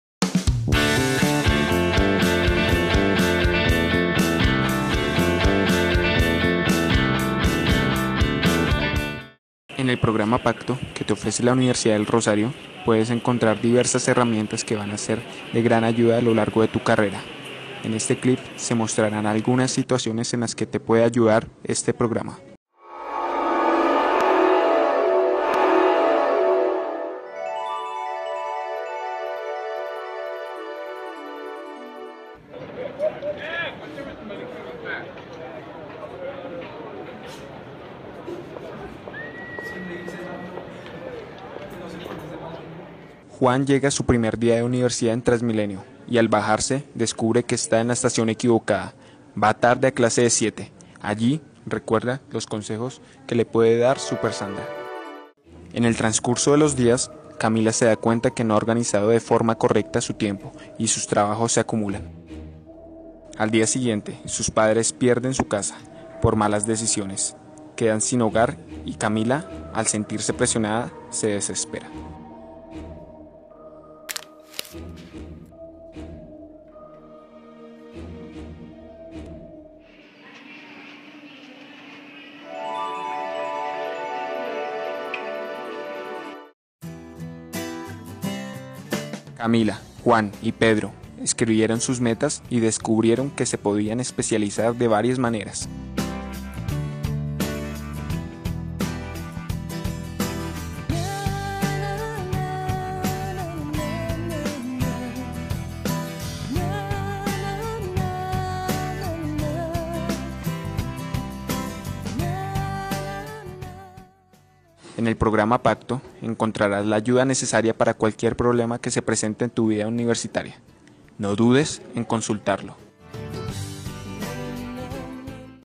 Un Pacto por
En nuestra Facultad nos reinventamos la inducción. ¡Aquí te mostramos los resultados!